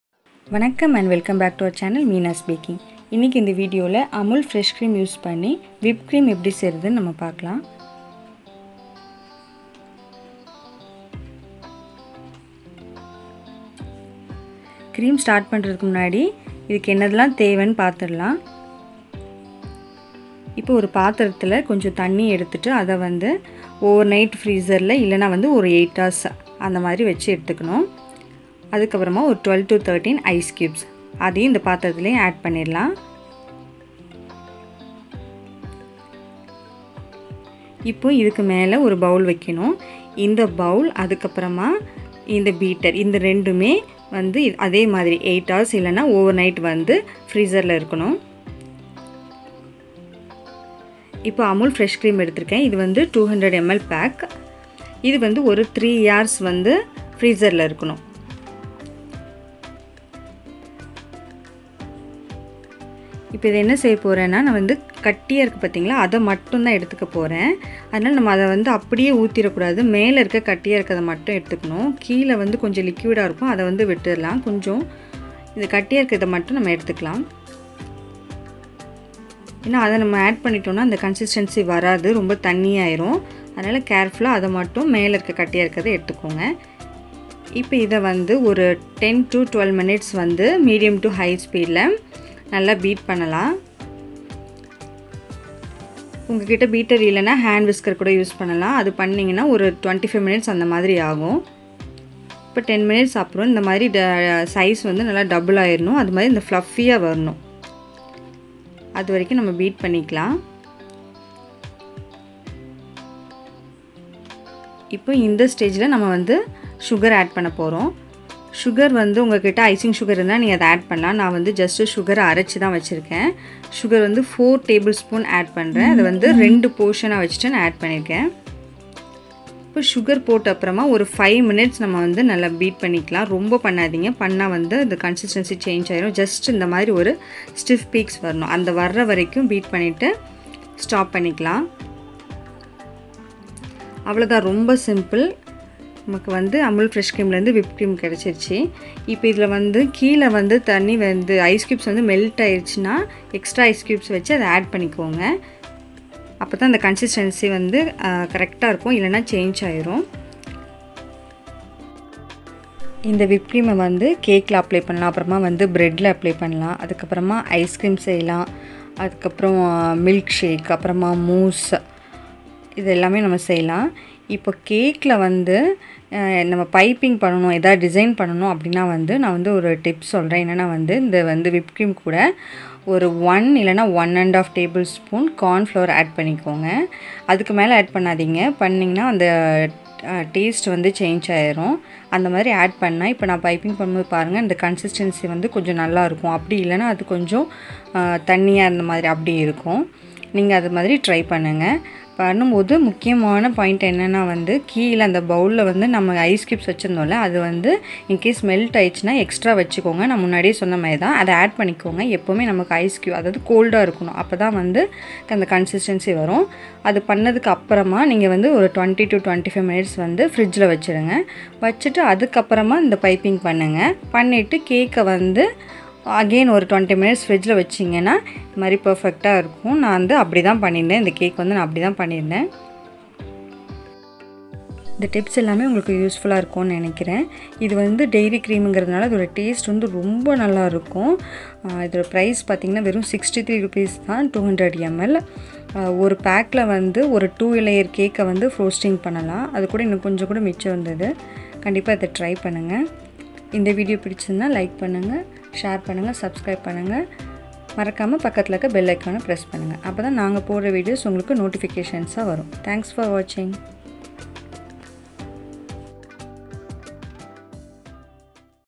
Whipped Cream from AMUL Fresh Cream in Tamil I Secret Ingredient for Perfect Piping
Amul Fresh Cream has been processed to give smooth, consistency and makes mouth watering preparations. Amul fresh cream sterilized at high temperature and packed aseptically to give safe, rich cream which stays fresh until opened. Made of fresh & Pure Milk cream. No preservative added.
Milk FAT: Min 25%, SNF: 6.8%, Moisture:68.2%

Amul Fresh cream is used to prepare fruit salad. Can be used to prepare Punjabi dishes. Used for preparing sweet meals. For making Ice-creams. Used in tea and Coffee.

This can also be used for Cake/Bread spread. This cream is not meant for Piping, but with a secret ingredient we have used you can can this cream even for Piping.